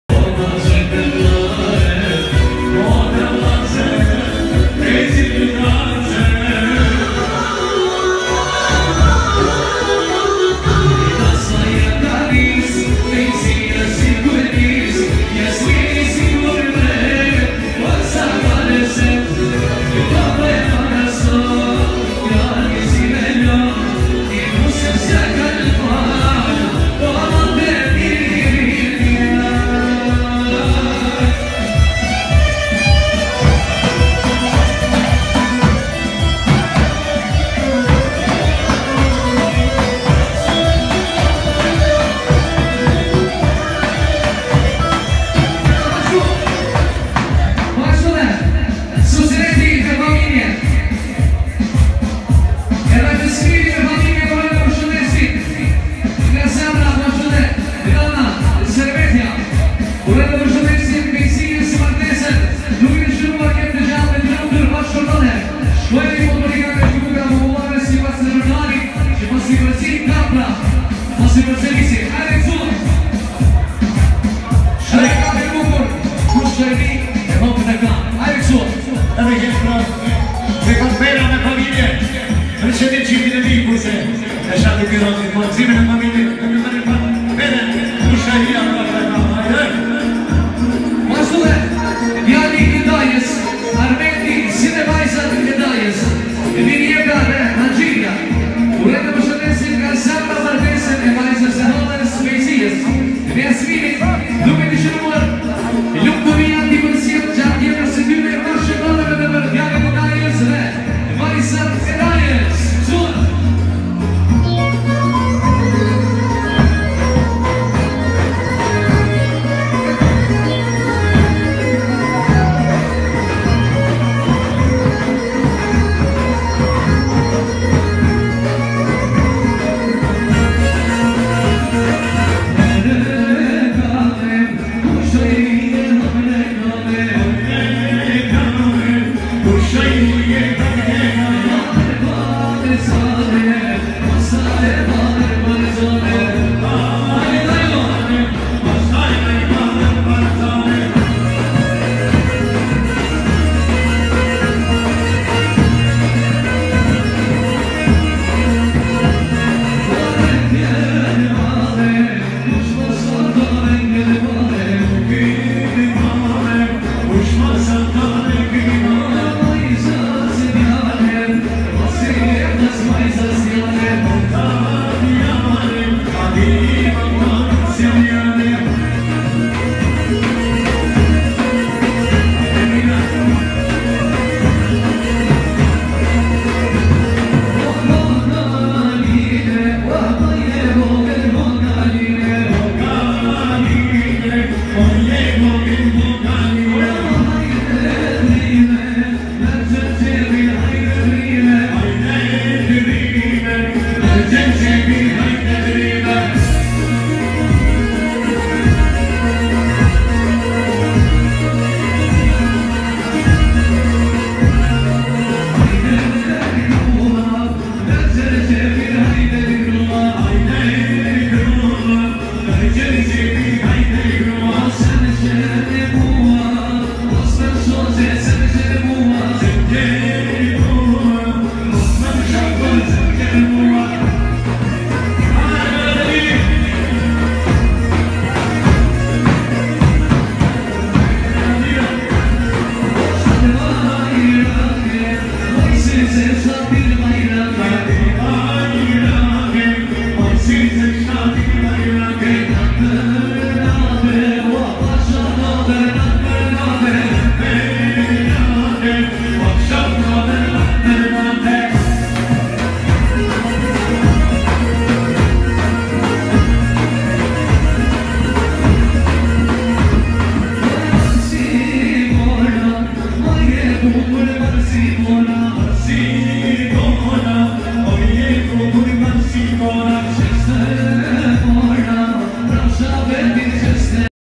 Ksi Dasmoreshave me vallzu kaq bukur mesiguri skeni pa kurre n jet
Ksi Dasmoreshave me vallzu kaq bukur mesiguri skeni pa kurre n jet sepse e ndezin krejt atmosferen kur hyjne ne valle.